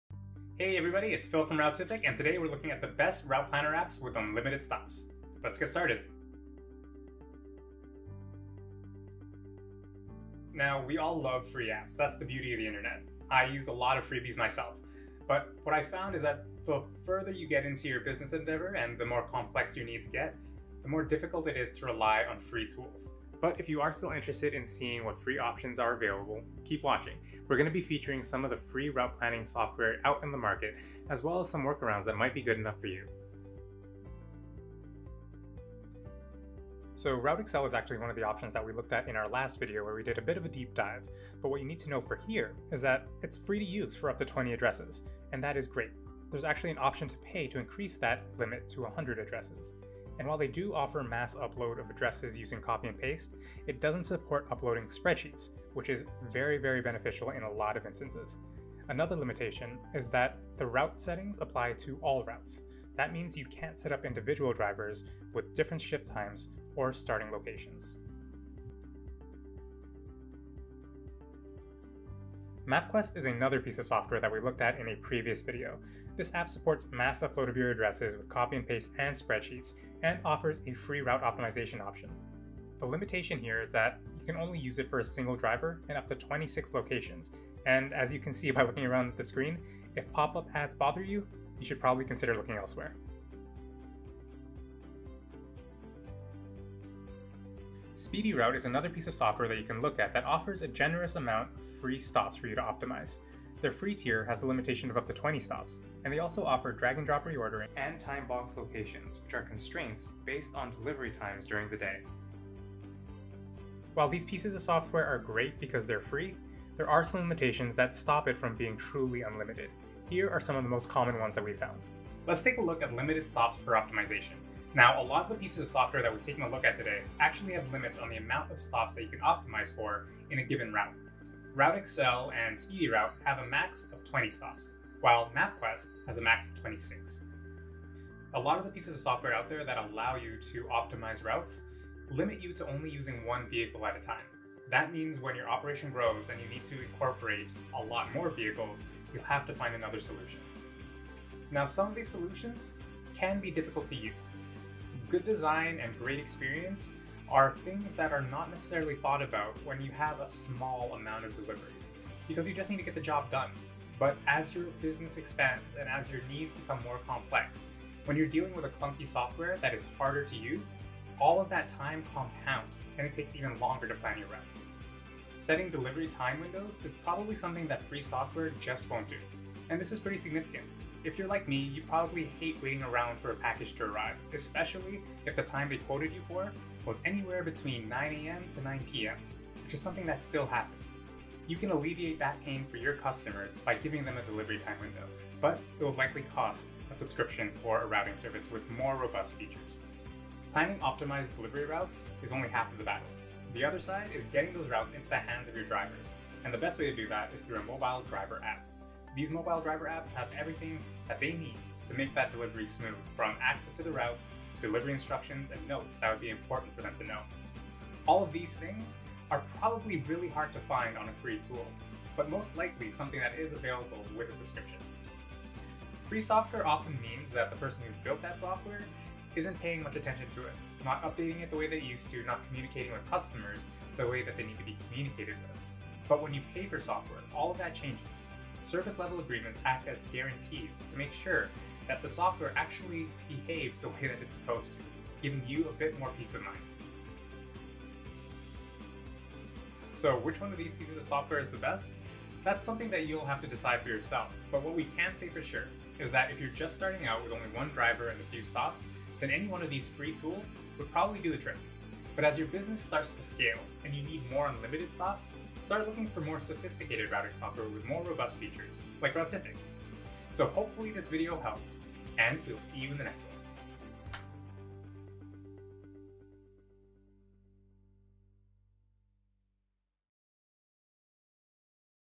Best Free Route Planning Software With Unlimited Stops
What are the best free route planning software with unlimited stops for deliveries? A quick guide to the trade-offs, advantages, and best practices. ⬇️

Chapters:
0:00 - Intro
00:23 - Free route planner apps with limitations​
1:56 - The limitations of free tools​

As your business scales, you might need routing software with more robust features. You can check out Routific free for 7 days.